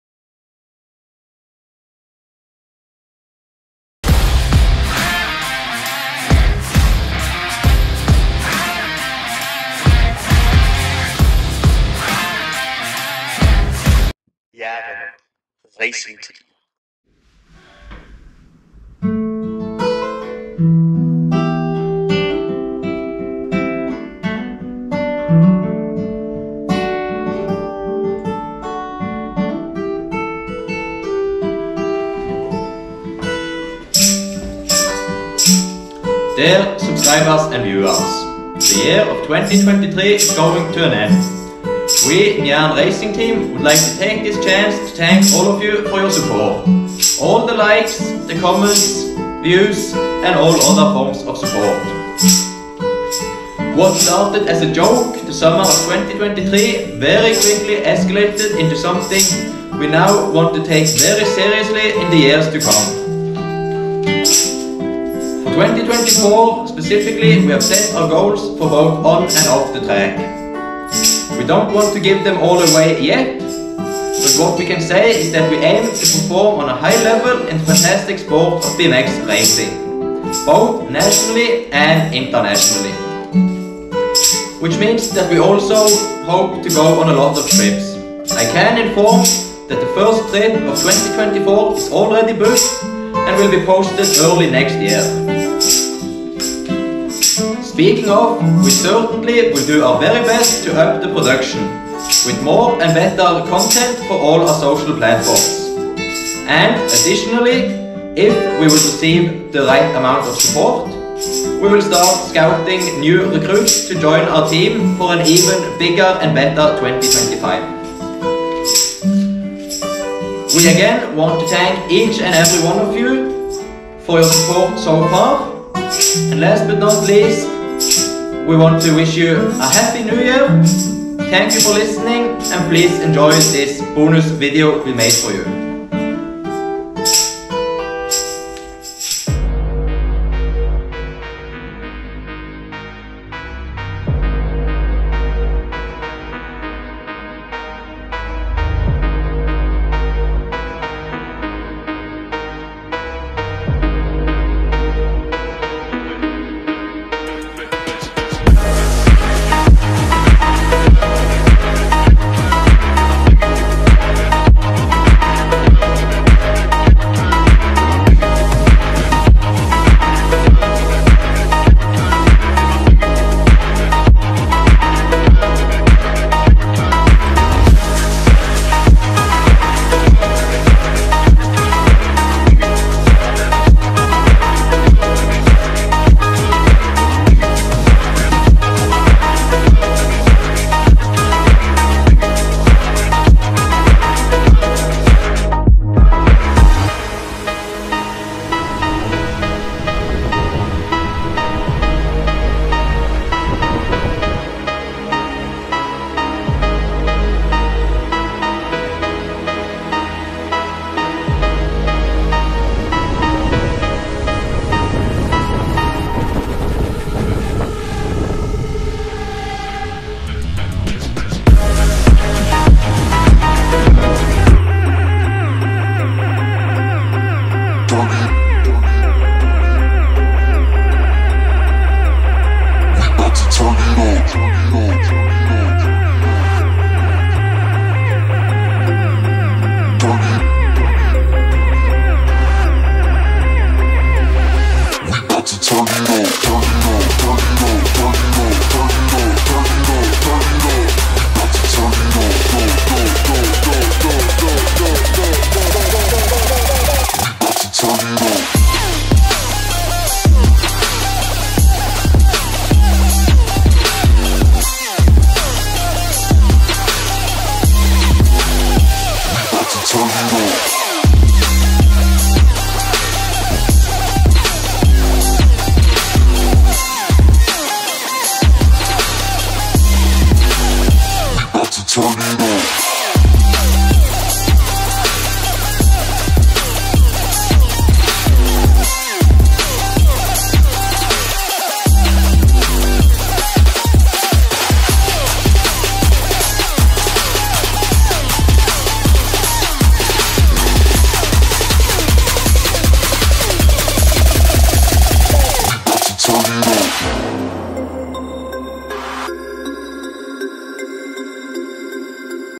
CRAZY New Year´s Celebration With JÆREN RACING TEAM
Jæren Racing Team celebrates the new year by testing our self-produced fireworks combined with some BMX cycling.
We in the Jæren Racing Team would like to thank all our supporters and wish everyone a happy new year!!